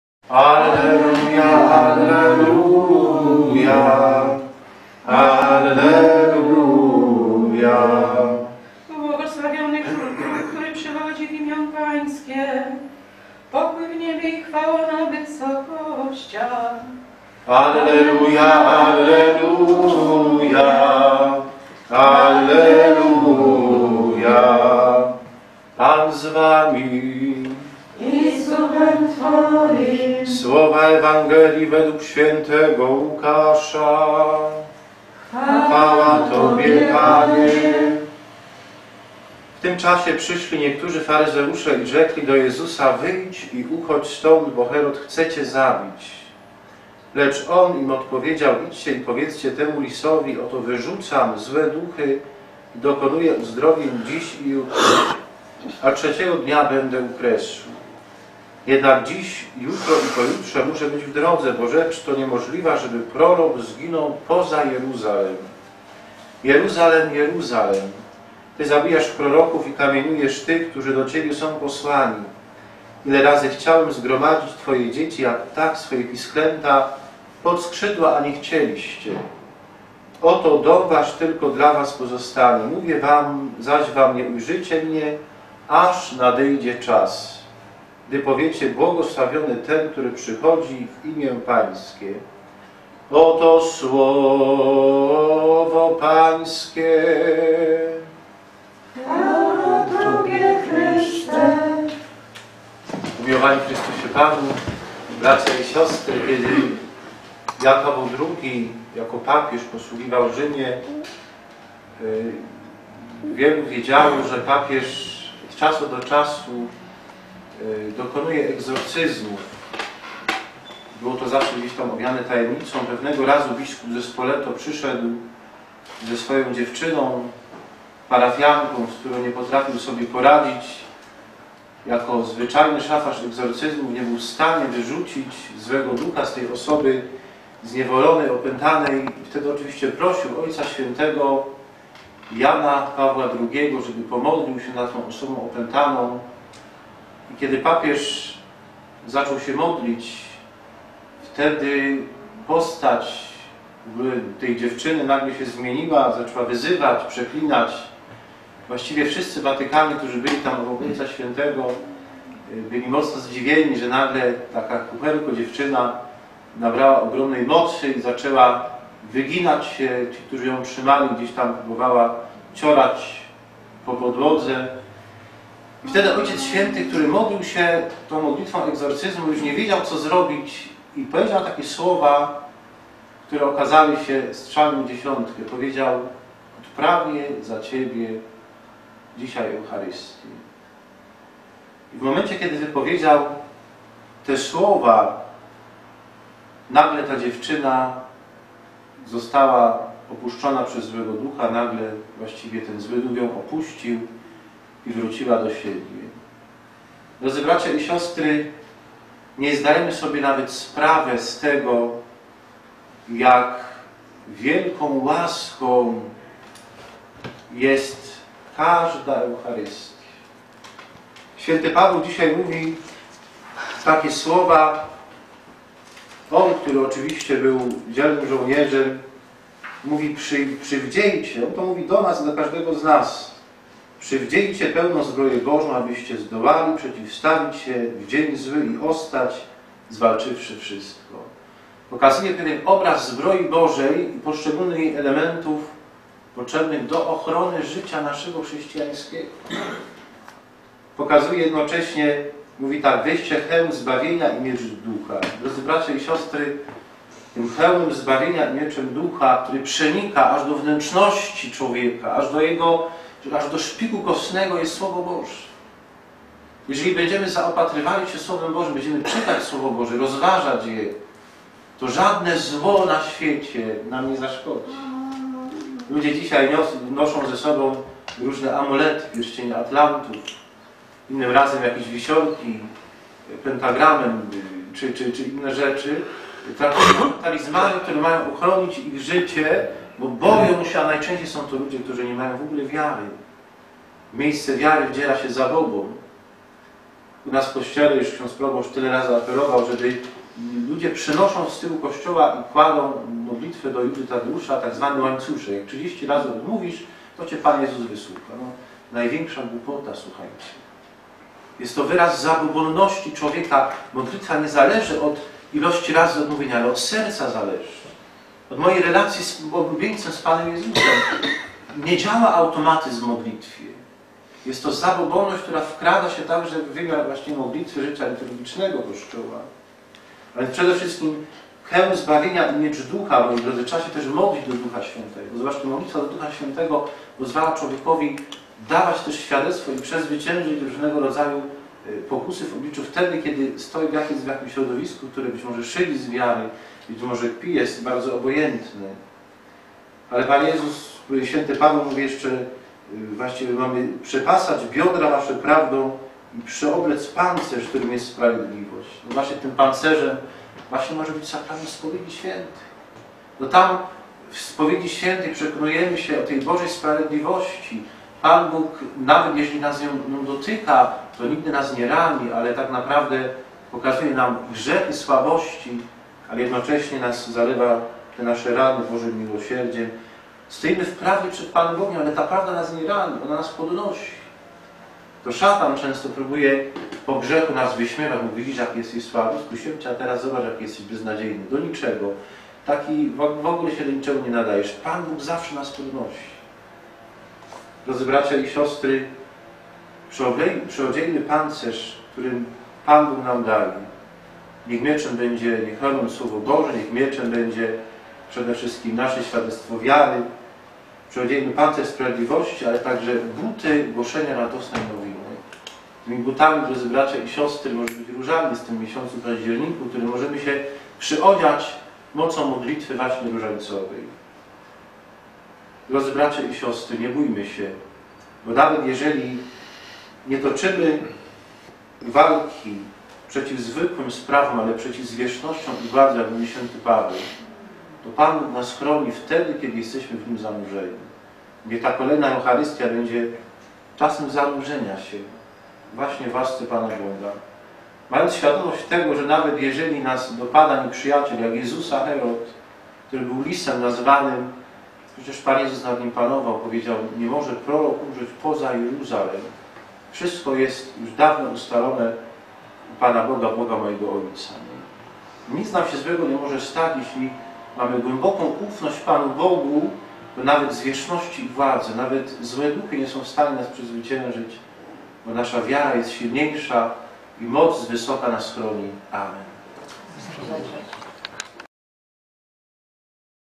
MSZA ŚWIĘTA W HOSPICJUM-CZWARTEK, 27 PAŹDZIERNIKA 2016r.
Ewangelię i homilię o wielkiej mocy i łasce Sakramentu  Eucharystii głosi ks. dr Marcin Niesporek.
Skuteczność modlitwy zależy od serca, od mojej relacji z Panem Jezusem. W modlitwie nie działa automatyzm ale otwartość serca na Łaskę Boga. Nic nam nie może się stać, gdy mamy ufność w Panu Bogu.